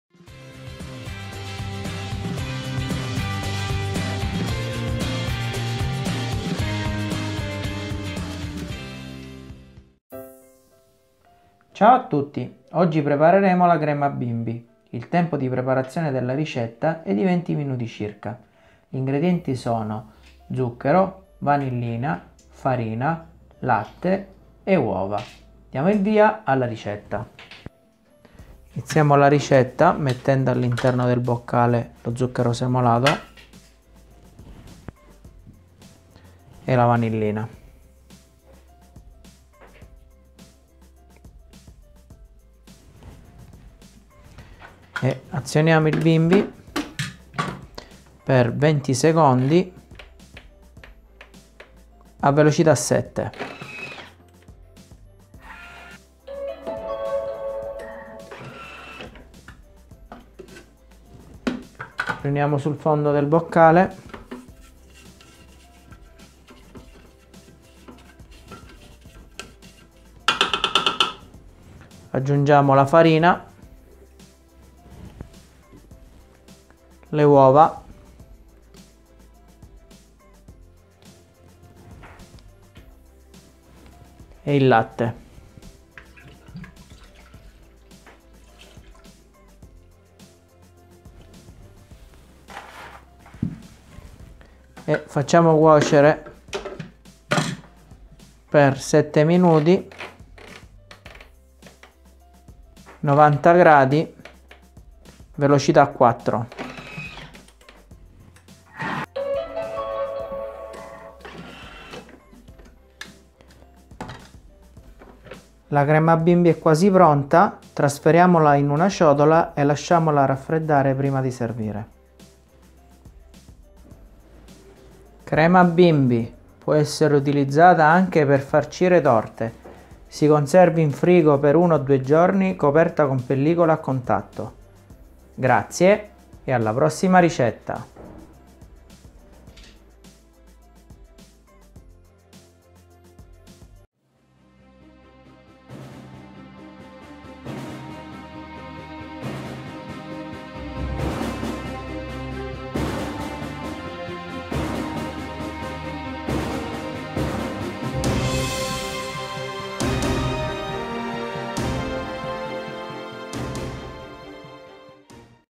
Crema bimby per TM6 TM5 TM31 TM21
Ingredienti:
100 g di zucchero semolato
2 bustine di vanillina
40 g di farina di tipo "00"
500 g di latte parzialmente scremato
2 uova